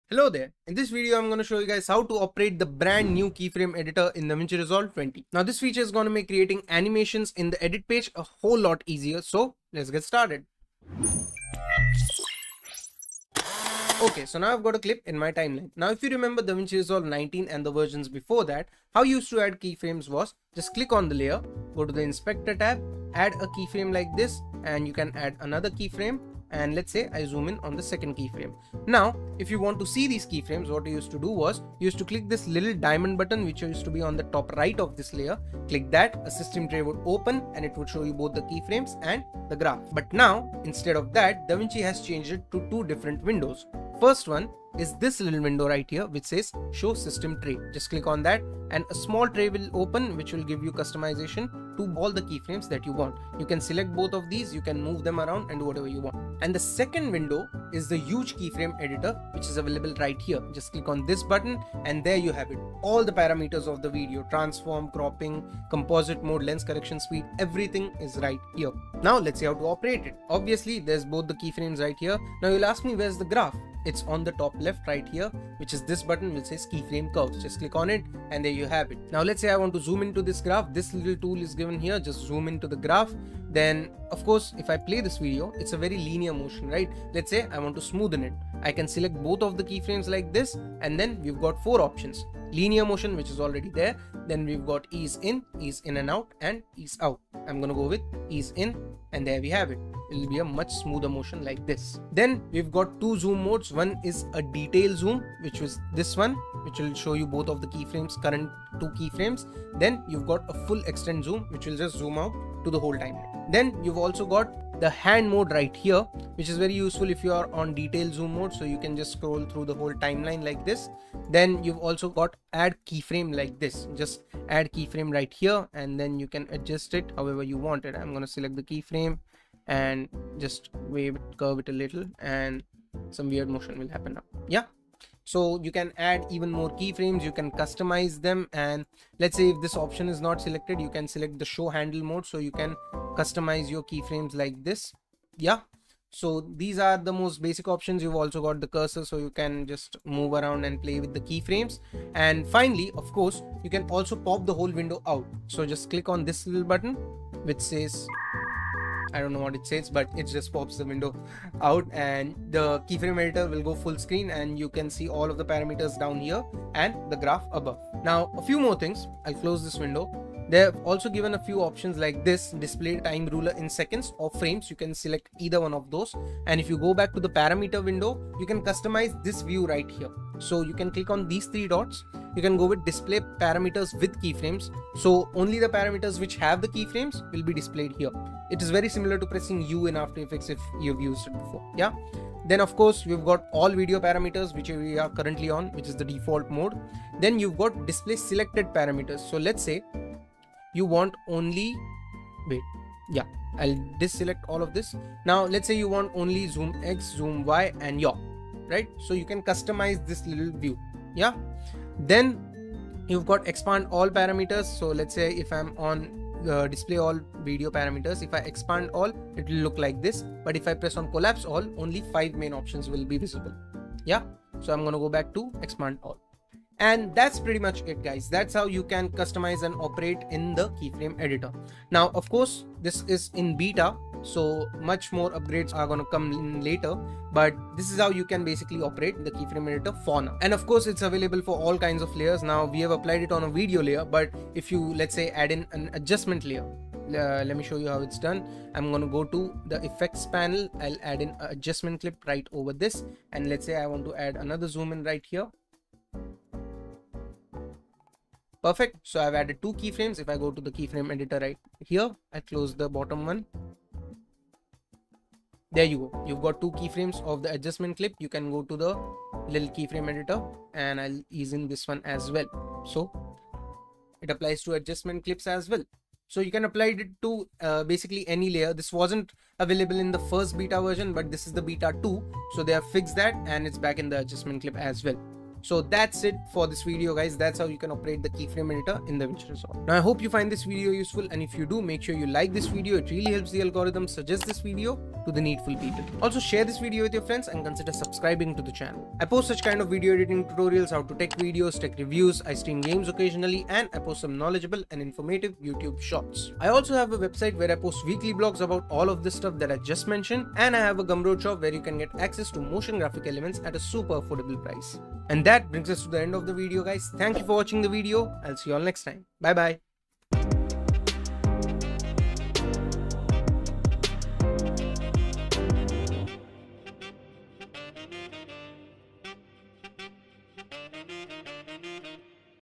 Game-Changing Keyframe Editor in DaVinci Resolve 20 | Complete Walkthrough
🚀 New in DaVinci Resolve 20: The Keyframe Editor!
In this video, I’ll walk you through how to use the brand new Keyframe Editor in DaVinci Resolve 20. Whether you're a beginner or an experienced video editor, this feature will help you create smoother animations, better motion graphics, and more control over your keyframes.

🔍 What you'll learn:
What the new Keyframe Editor is
How it differs from previous versions
Step-by-step guide to animating using keyframes

✅ Perfect for:
Editors upgrading to DaVinci Resolve 20
Creators who want better animation control
Anyone learning video editing in 2025

Chapters :
00:00 Introduction
00:13 How it was before
00:39 Keyframe Tray in Davinci Resolve 20
00:56 Keyframe Editor Window in Davinci Resolve 20
01:12 Keyframe Graph Editor View in Davinci Resolve 20
01:18 Customizations and Features of Keyframe Editor in Davinci Resolve 20